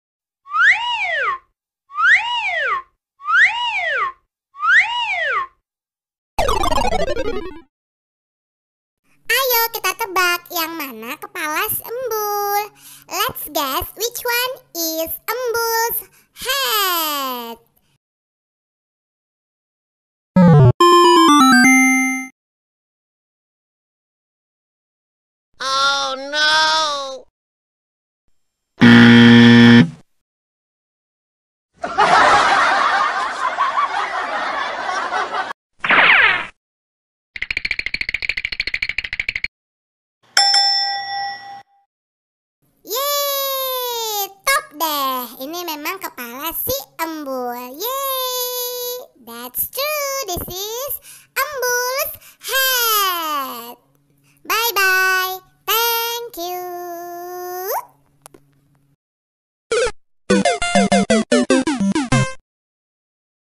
Let's find Green rabbit's head Embul😱😂😜. Yang mana kepala si embul🤣
halo teman teman Serenity Diary, yuk ditonton nih video terbaru dari Kami, bernyanyi, bermain, dan menebak gambar. Kali ini ada karakter green rabbit atau si Embul nih,.
Ya sudah yah, langsung ditonton saja ya, semoga teman-teman suka.

Terima Kasih.. bye bye...

Green rabbit tiktok, fat rabbit, si embul, funny rabbit, embul lucu